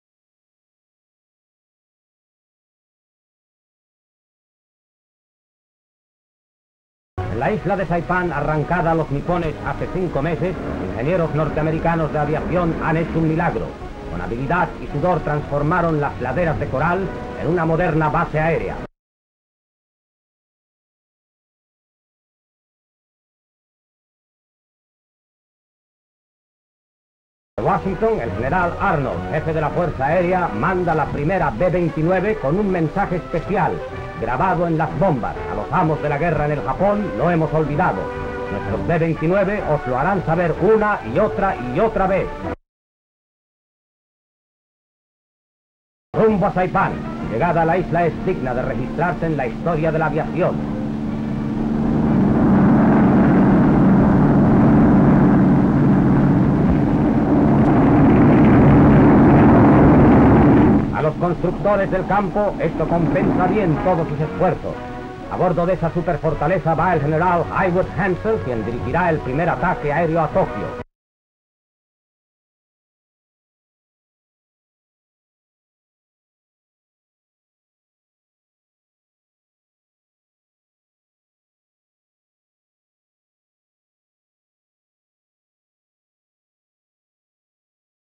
Soldados norteamericanos contruyen base en la isla de Saipan 1945
Super-Forts carry war to Tokyo! La isla de Saipan arrasada a los nipones hace 5 meses, ingenieros norteamericanos de aviación han hecho un milagro, con habilidad y sudor transformaron las praderas de coral en una moderna base aérea. En Washington D.C. el general Henry Harley Arnold, jefe de la Fuerza Aerea manda la primera B-29 con un mensaje especial grabado en las bombas: -"A los amos de la guerra en el Japon no hemos olvidado. Nuestros B-29 os lo harán saber una y otra, y otra vez". Rumbo a Saipan, llegada a la isla insignia de registrarse en la historia de la aviación. A los constructores del campo estos expresan bien todos sus esfuerzos. A bordo de esa superfortaleza va el general Haywood S. Hansell, quien dirigirá el primer ataque aereo a Tokio.
Detalle: Vistas generales de los trabajos realizados en la isla con la construcción de la base militar. Hombres trabajando en la pavimentación de la pista. Imagen del general Arnold reuniéndose con la tripulación del avión B-29. Imagen de la bomba con el mensaje. Vistas generales del avión en vuelo hacia la isla de Saipan. Imagen del avión aterrizando en la pista. Imagen del general Haywood S. Hansell saludando al comandante de la base aérea.
Fecha: 21/7/1945
Duración: 1 minuto 9 segundos
Código del film: B-06629